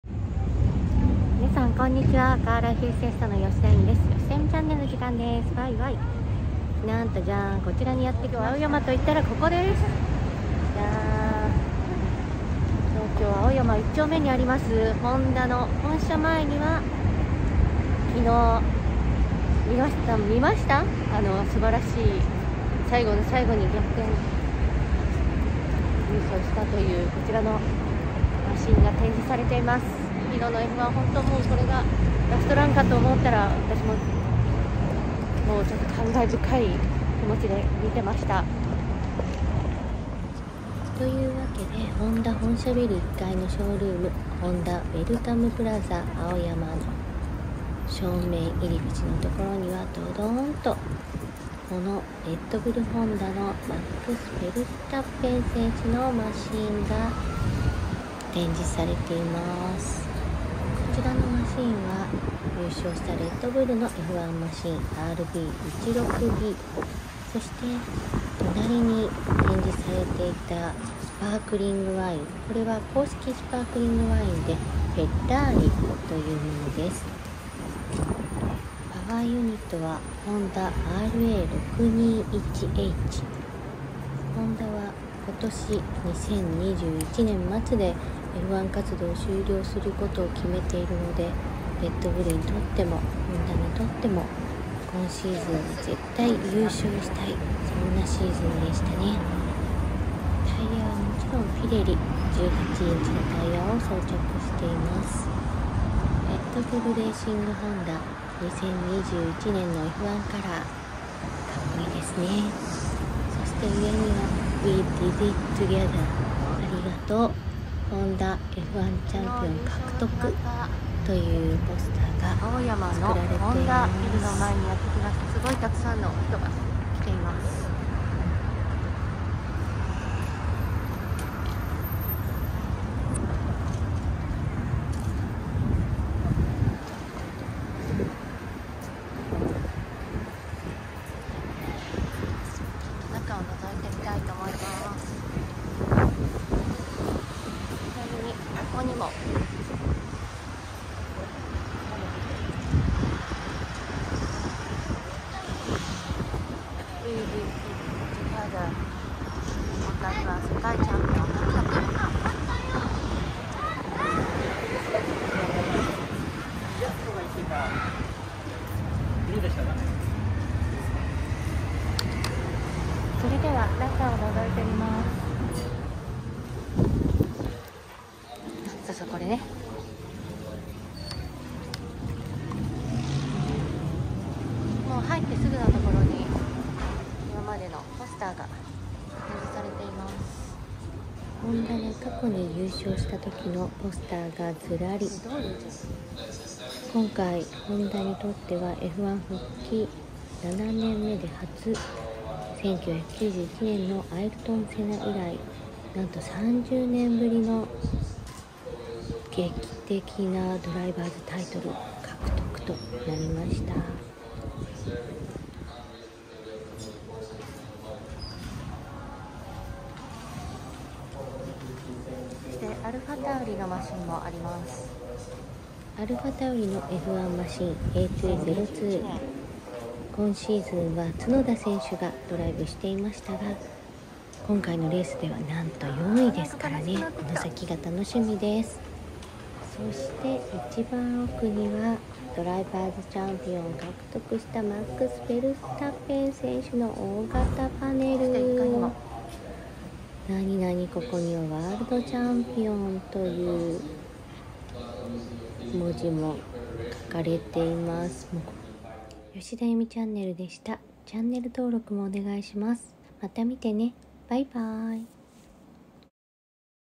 ホンダF1最終年、悲願のドライバーズチャンピオン獲得！最終戦アブダビGP決勝翌日のホンダウェルカムプラザにレッドブルホンダ、アルファタウリのマシン！
F1レッドブルホンダ　フェルスタッペン選手　悲願の逆転優勝記念の特別展示が、現在、東京青山のウェルカムプラザ青山にて開催中

レッドブルホンダF1やアルファタウリのF1マシンなど

最終ラップに起きた奇跡✨に大興奮した方は、こちらに足を運んでみとはいかがですか？

平日なら空いてます

2021年12月28日まで開催